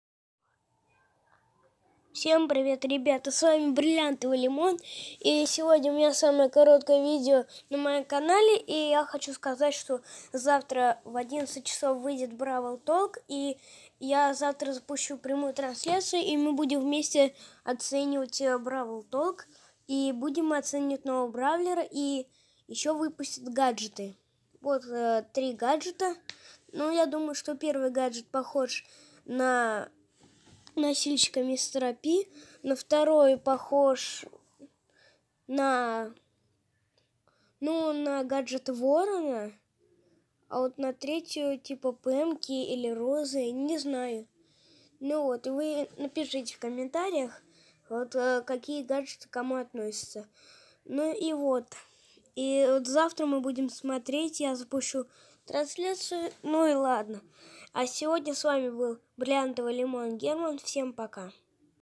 💎СРОЧНО! ЗАВТРА БРАВЛ ТОЛК. НА СТРИМЕ ЗАВТРА ОЦЕНИМ.💎
Завтра я выпущу стрим оцениваем бравл толк и ждем обновы. Стирим начнется в 11:50 15 минут. Весь бравл толк